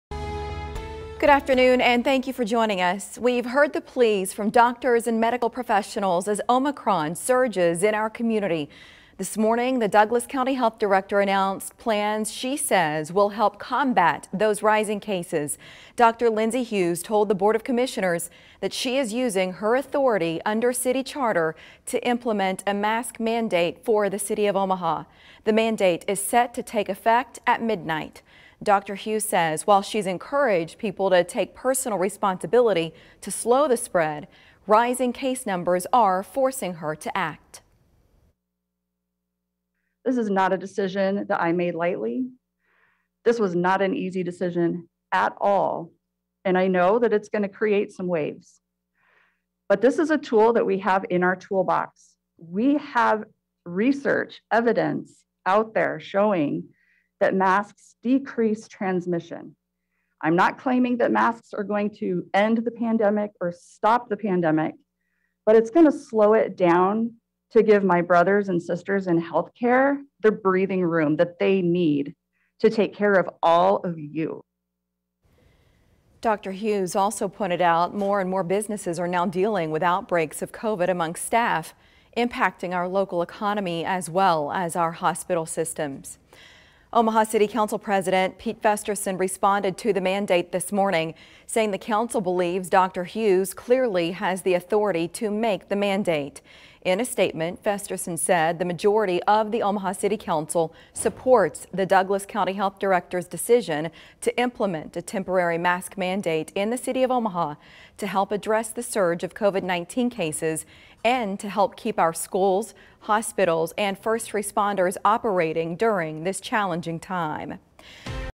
Health Director institutes mask mandate: 'I can't stand by and not do everything that we can'
Douglas County Health Director Dr. Lindsay Huse said Tuesday that she intends to implement a mask mandate in response to rising COVID cases.